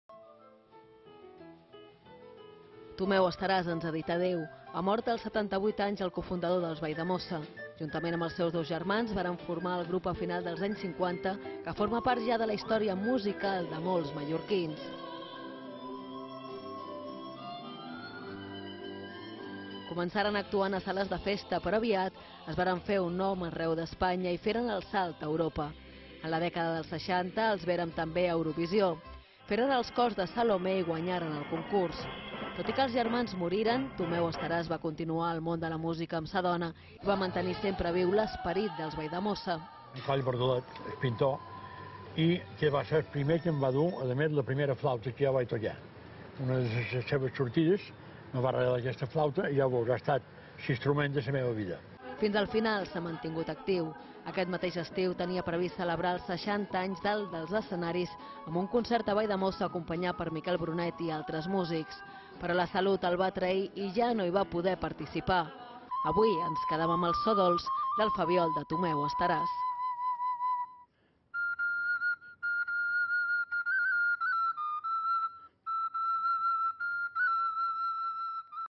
Mor Tomeu Estaràs, un dels fundadors dels Valldemossa
I avui capvespre s’ha confirmat la mort del músic Tomeu Estaràs. Va ser cofundador dels Valldemossa, el mític grup sorgit a finals dels anys 50 i que forma part ja de la història musical de Mallorca.

Tomeu Estaràs ens ha dit avui adeu. Ha mort als 78 anys el cofundador dels Valldemossa. Juntament amb els seus dos germans, varen formar el grup a finals dels anys 50 que forma part ja de la història musical de molts mallorquins.

Començaren actuant a sales de festa, però aviat es varen fer un nom arreu d’Espanya i feren el salt a Europa. En la dècada dels 60, els vérem també a Eurovisió. Feren els cors de Salomé i guanyaren el concurs.
Tot i que els seus germans moriren, Tomeu Estaràs va continuar al món de la música amb sa dona i va mantenir sempre viu l’esperit dels Valldemossa.

Fins al final, s’ha mantengut actiu. Aquest mateix estiu tenia previst celebrar els 60 anys dalt dels escenaris amb un concert a Valldemossa acompanyat per Miquel Brunet i altres músics. Però la salut el va trair i ja no hi va poder participar. Avui ens quedam amb el so dolç del flabiol de Tomeu Estaràs.